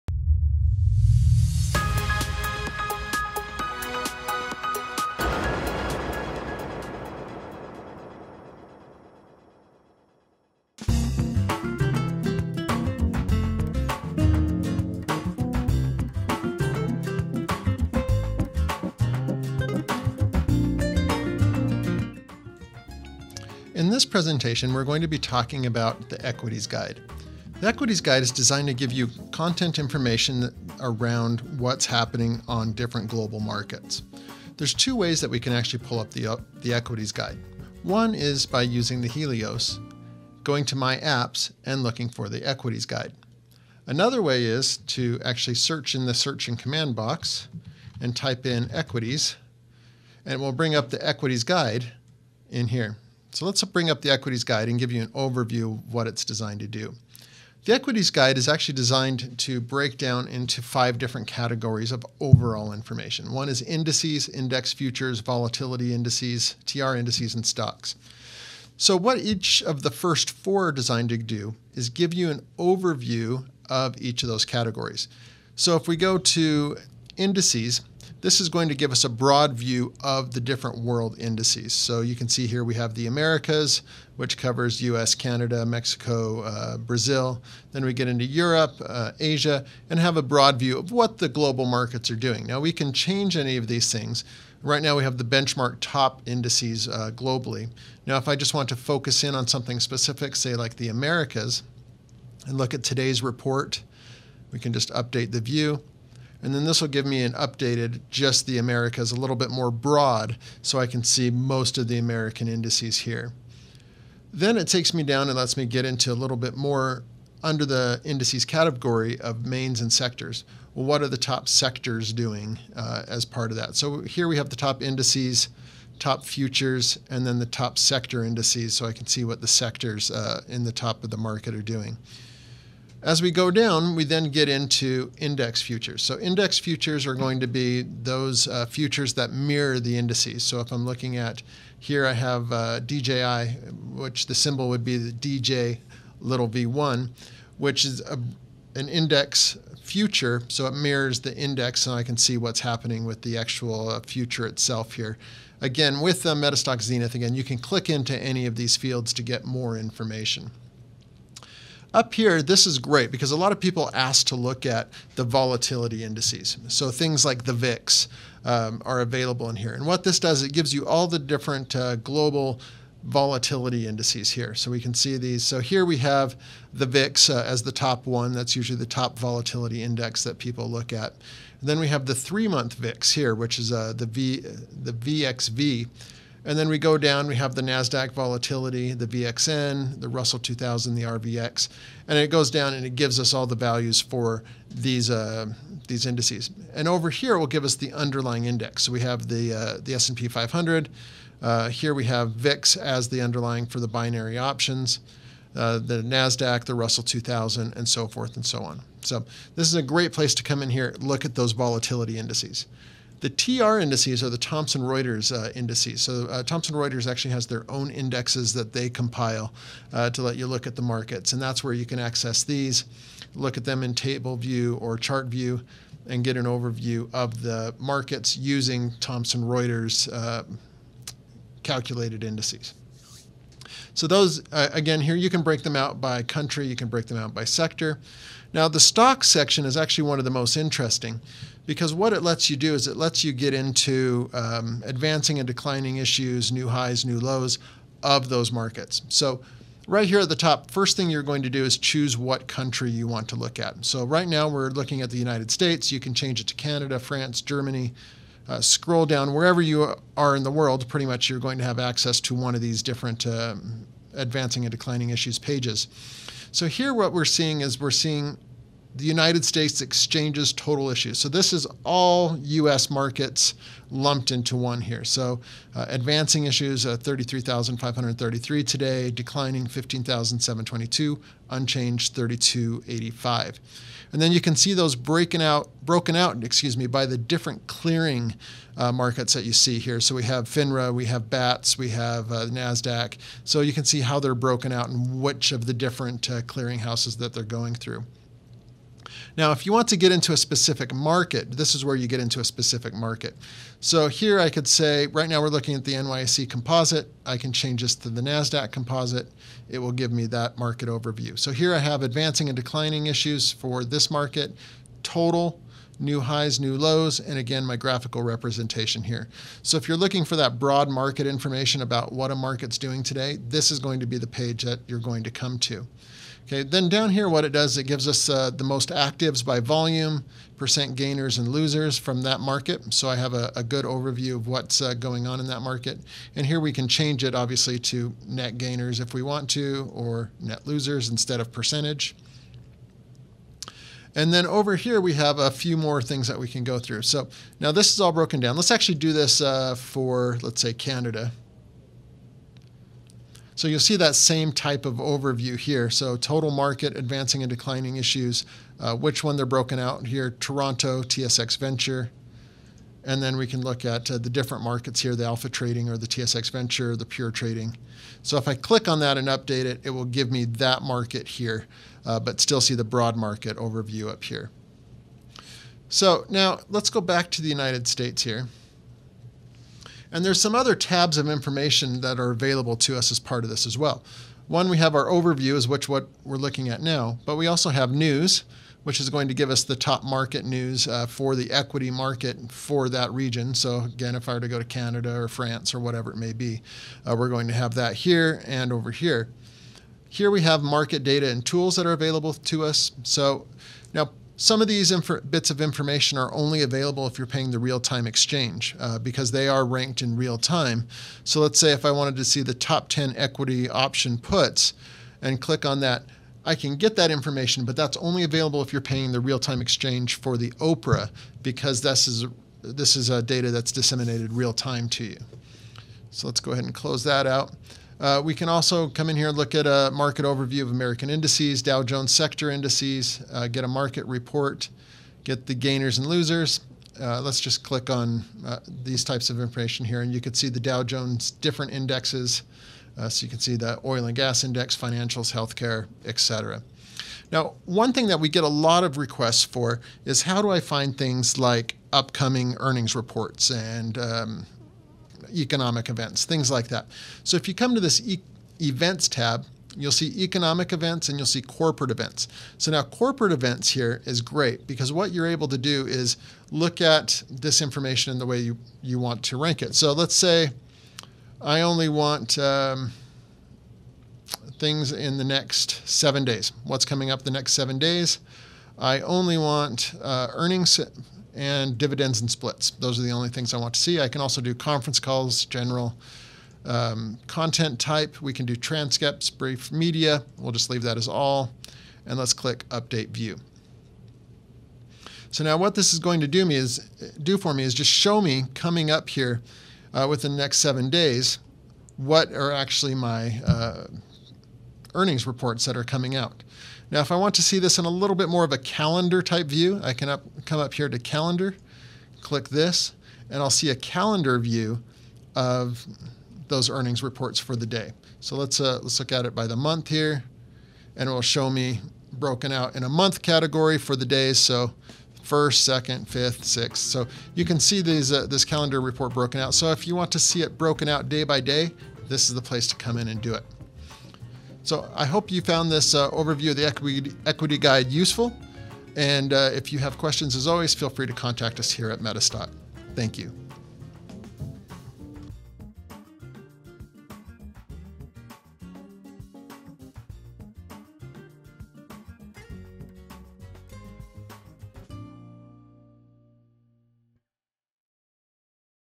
How to Use the Equities Guide in XENITH
In this presentation, Kelly Clement shows you how to use the Equities Guide in XENITH Real-Time Market Data and News.